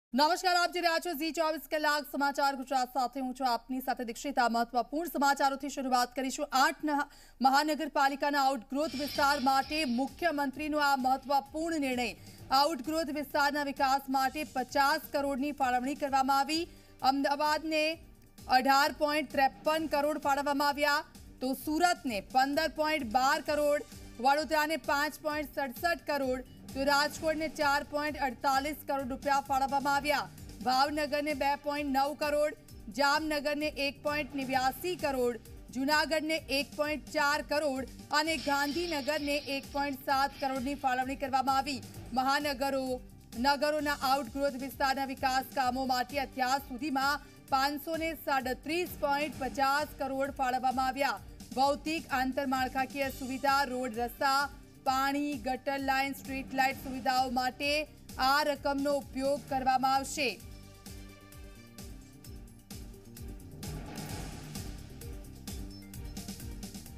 Guj govt approves Rs 50 cr grant for outgrowth areas of municipal corporations&municipalities bodies
Gujarat govt approves Rs 50 cr grant for ‘outgrowth areas’ of municipal corporations and municipalities bodies | Zee News

8 મહાનગર પાલિકાના આઉગ્રોથ વિસ્તાર માટે મુખ્યમંત્રીનો મોટો નિર્ણય
આઉટગ્રથ વિસ્તારના વિકાસ માટે 50 કરોડની ફાળવણી
અમદાવાદને 18.53કરોડ, સુરતને 15.12 કરોડ, વડોદરાને 5.67 કરોડ, રાજકોટને 4.48 કરોડ, ભાવનગરને 2.09 કરોડ, જામનગરને 1.89 કરોડ, જૂનાગઢને 1.04 કરોડ, ગાંધીનગરને 1.07 કરોડની ફાળવણી
મહાનગરો-નગરોના આઉટગ્રોથ વિસ્તારના વિકાસ કામો માટે અત્યાર સુધીમાં 537.50 કરોડ ફાળવાયા
ભૌતિક આંતરમાળખાકીય સુવિધા-રોડ-રસ્તા, પાણી, ગટર લાઇન, સ્ટ્રીટલાઇટ સુવિધા માટે આ રકમનો ઉપયોગ કરાશે